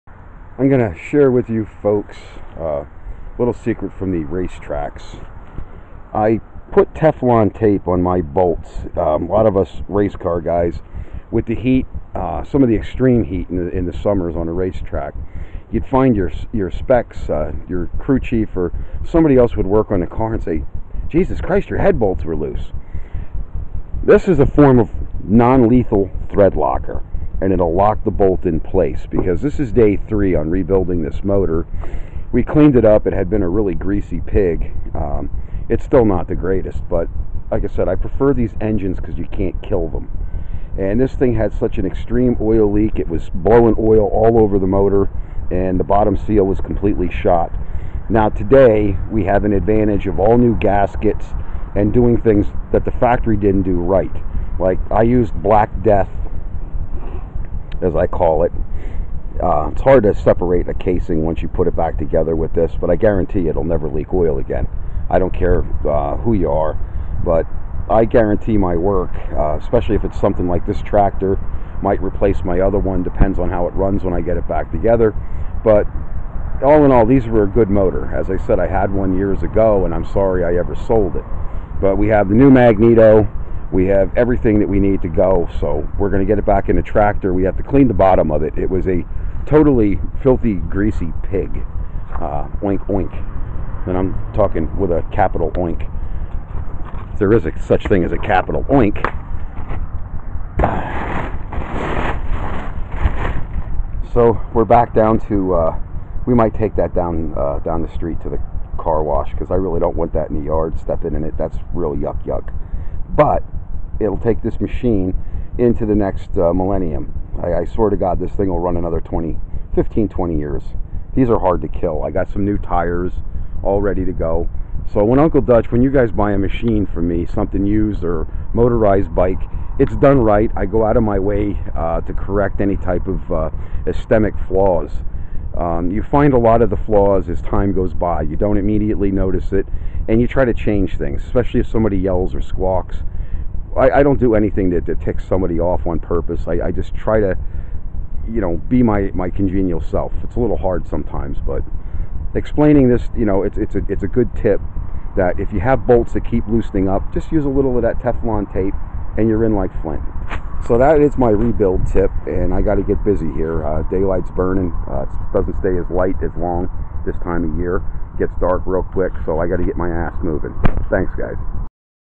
HARLEY BEE MOTORIZED BICYCLES...
This is Day 3 of rebuilding an old gem of a riding mower... No shitty plastic  parts and straight forward on how to resurrect your own oldy but  goodie...  This particular tractor had the BETTER PEERLESS transaxle than most of the MTD machines of the day and no plastic crap at all to deal with... All a straight forward rebuild it is day 3 of a resurrection process folks... Still not running but getting there...